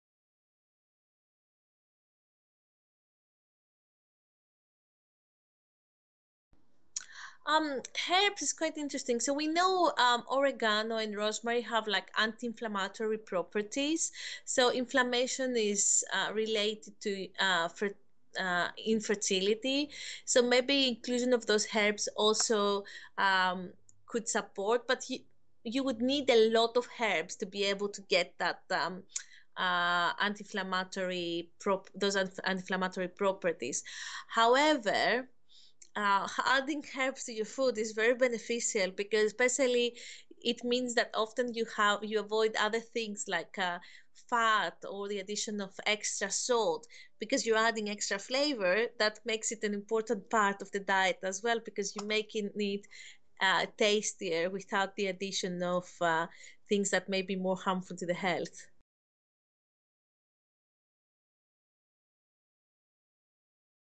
What herbs increase fertility?
Xanthi Maragkoudaki, Lecturer in Nutrition and Public Health at University of Westminster